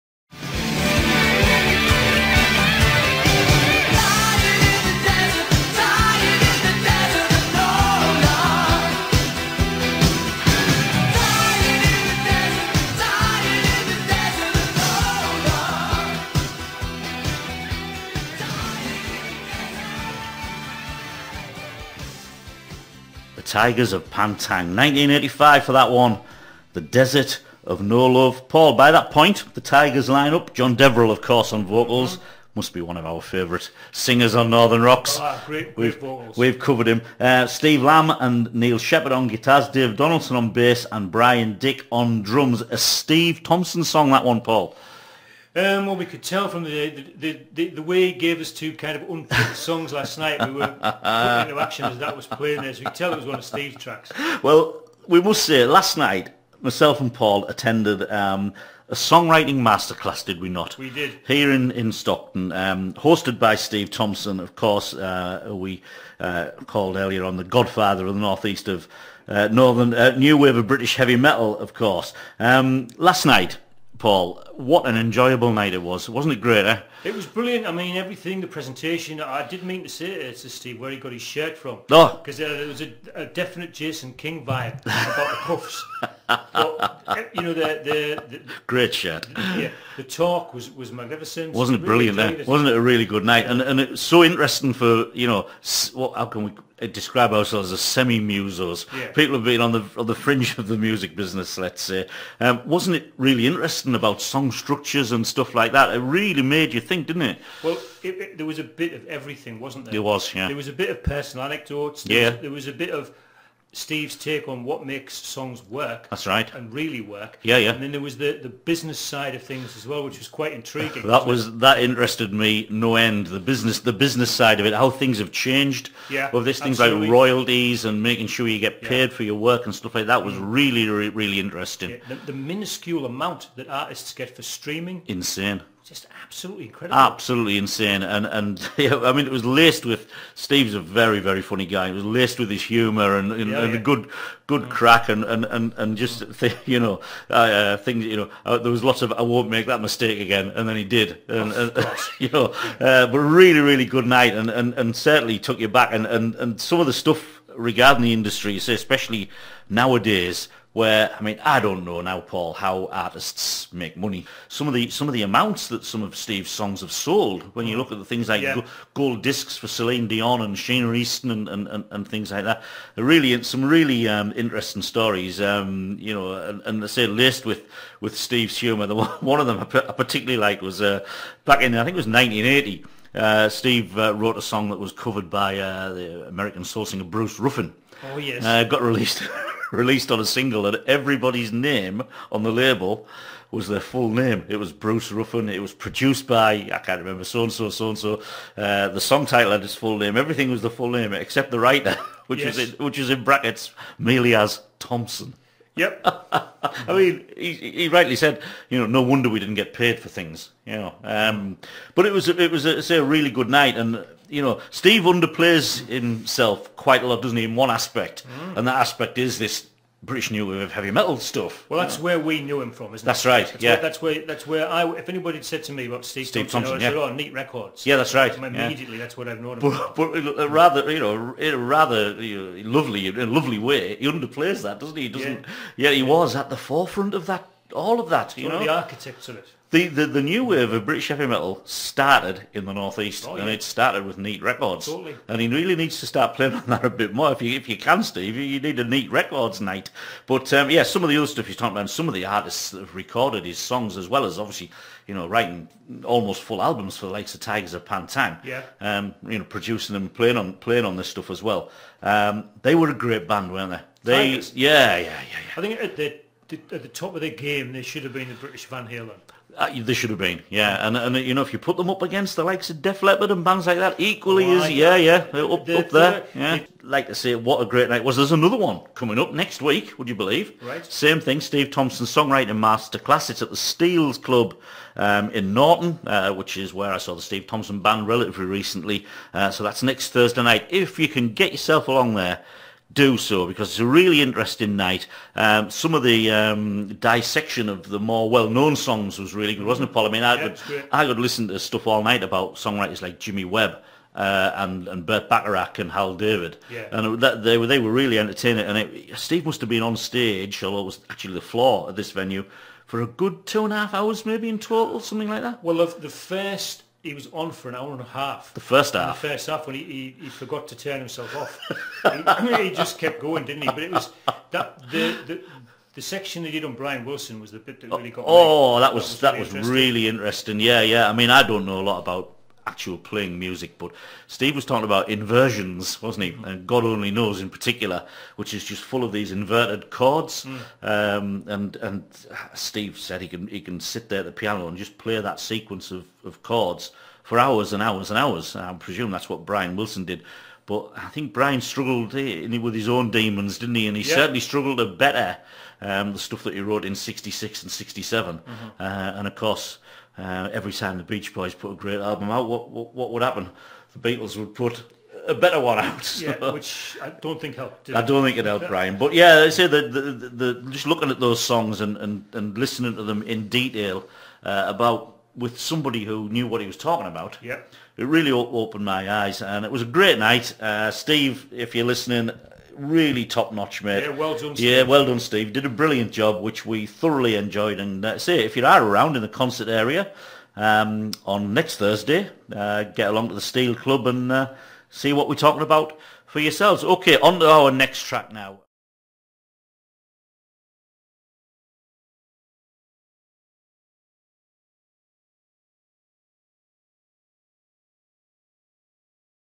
Songwriting Masterclass - Steve Thompson
Review of my Songwriting Masterclass from last week The Green Room, Stockton on Radio Stockton